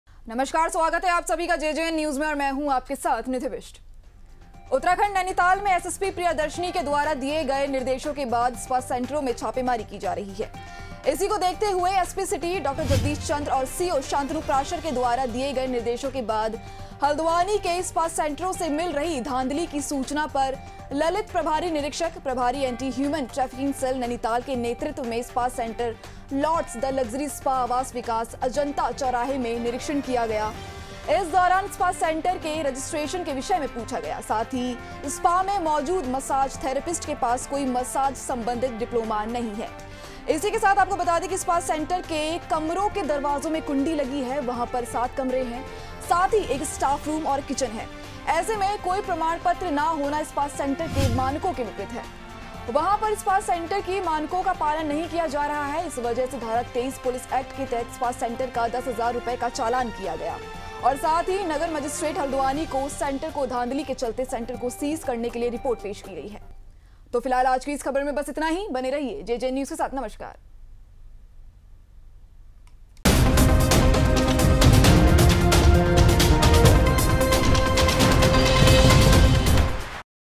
काटा 10 हज़ार का चालान, सेंटर को सीज़ करने की रिपोर्ट भी की पेश|| JJN News
Research/Script - Awantika Bisht
Anchor - Nidhi Bisht
Editor - Renu Sunita

JJN News Nainital,
JJN News,